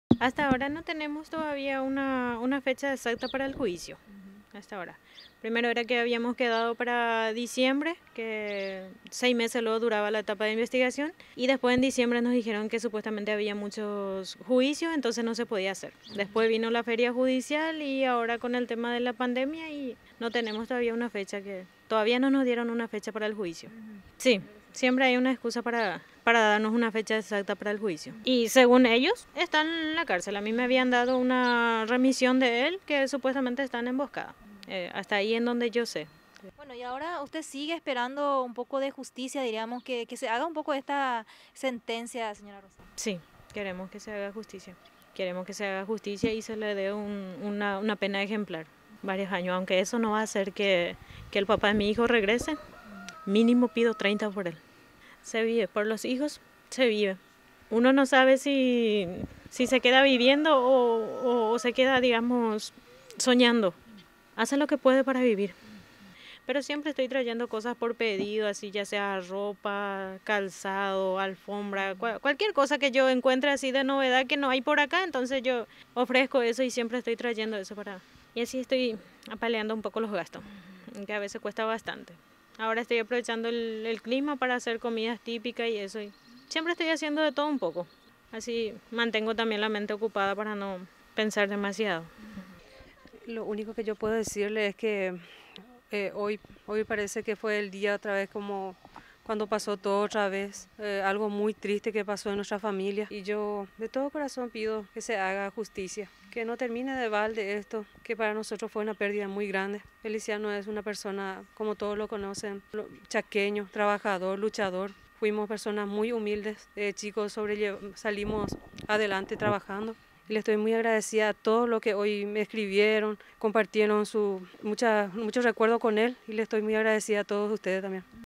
A UN AÑO DEL ASESINATO DE FELICIANO MARTÍNEZ SUS FAMILIARES AÚN NO ENCUENTRAN JUSTICIA
A un año de aquella fatídica noche del 05 de junio del año 2019 en donde la vida del periodista deportivo Feliciano Martínez fuera arrebatado a disparos, los familiares aún no encuentran justicia terrenal, lamentan que el proceso judicial avance de forma tan lenta y aguardan condena ejemplar para Ángel Albino Arriola Enciso, sindicado como el autor del crimen.